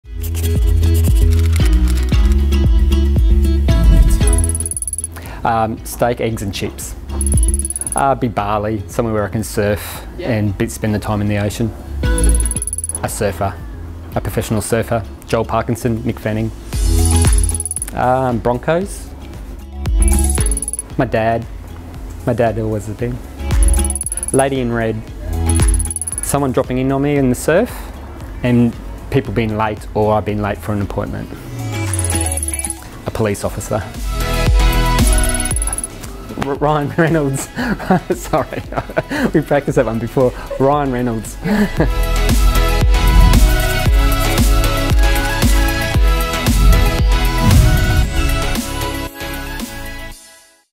Get To Know Mark Tebbutt
Mark Tebbutt was born and bred on the Sunshine Coast. A natural affinity with the ocean, Mark enjoys watching the sunrise from his board whilst waiting for the next wave. Selling and promoting the glorious lifestyle and community of the Sunshine Coast is something he does with both ease and passion.

"Assisting client's to achieve their property goals is hugely rewarding and a role I do not take on lightly; I'm happy to do the hard work required to facilitate a smooth, stress-free experience."